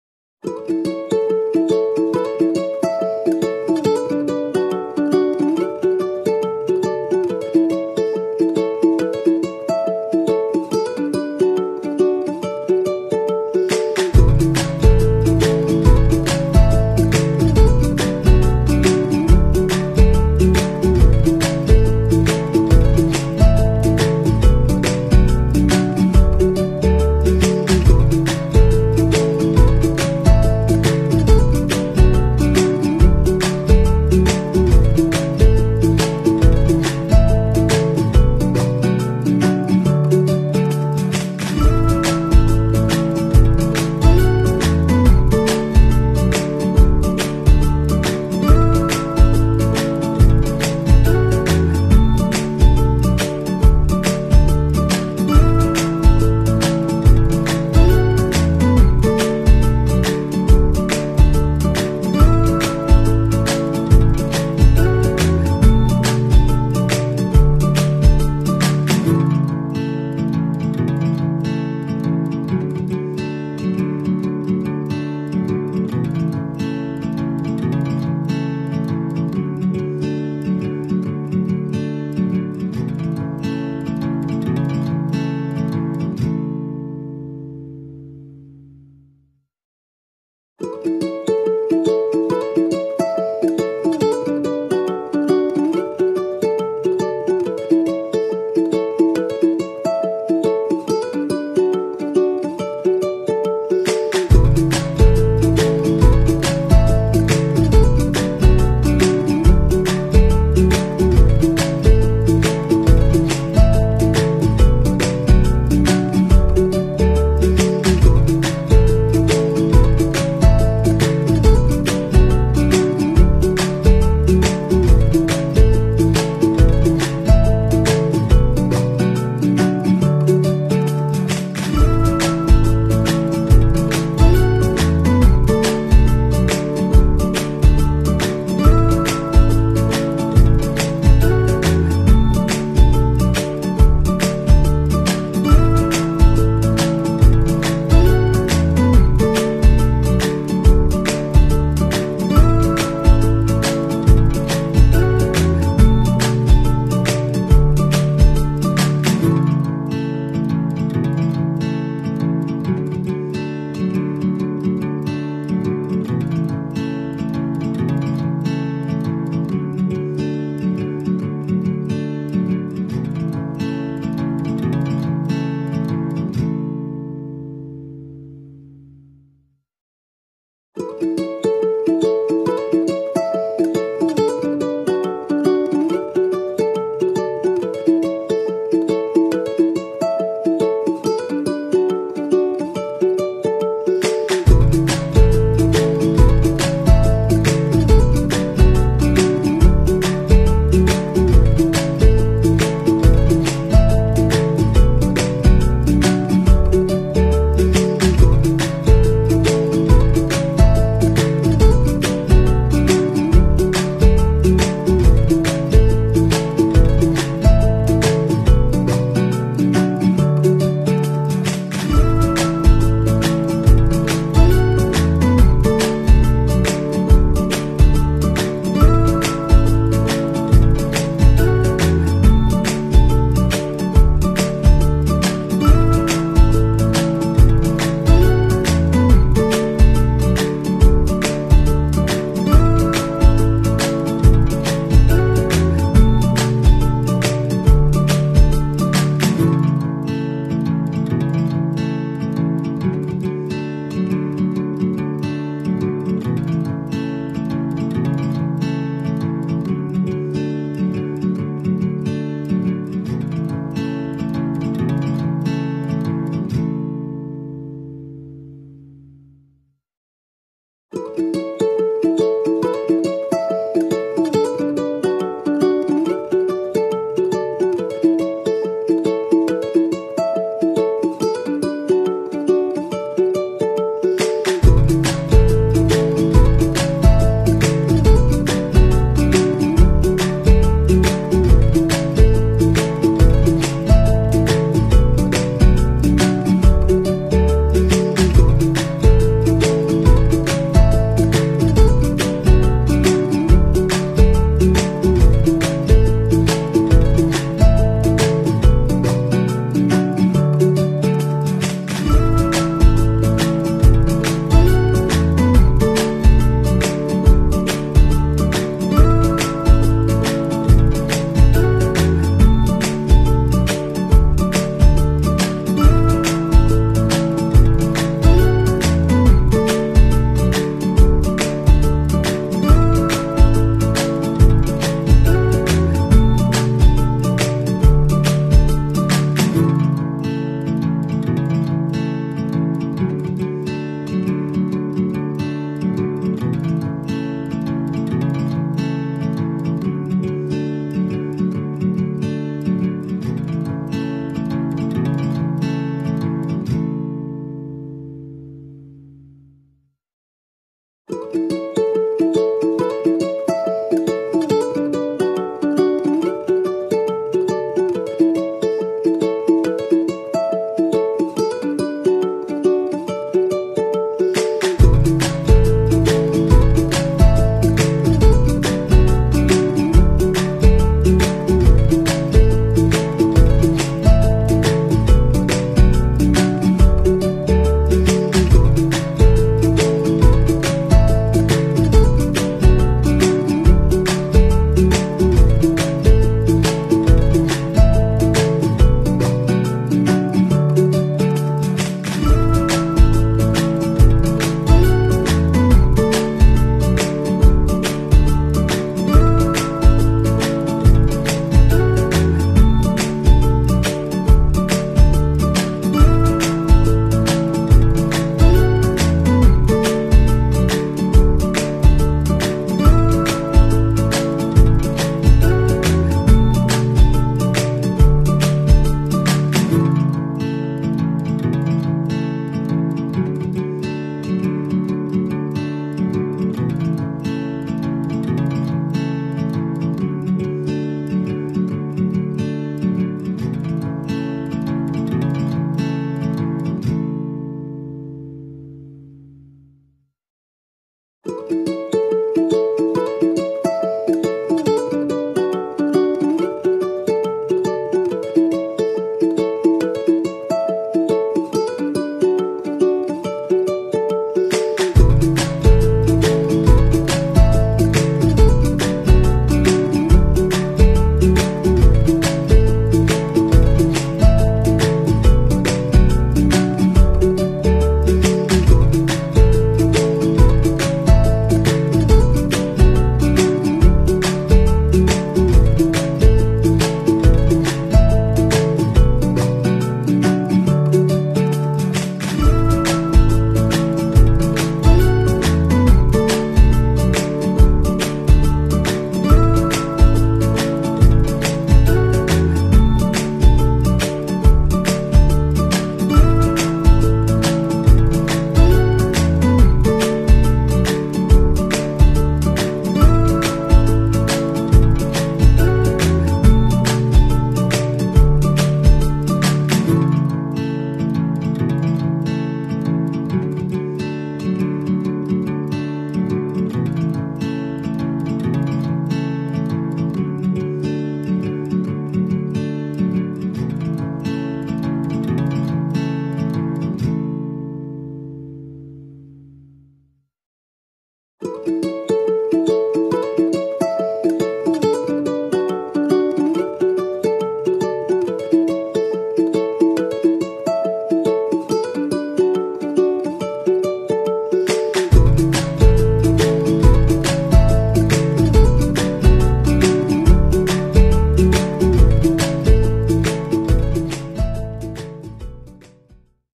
رسم شعر ولد بالرصاص
عندك موهبه اولا هتبقي فنان محترف
اول حلقه من رسم العنين
خطوه خطوه

I draw this eye with FABERCASTELL colors
Thank you for your time watching my video
Don't forget to click on the notification button 🥰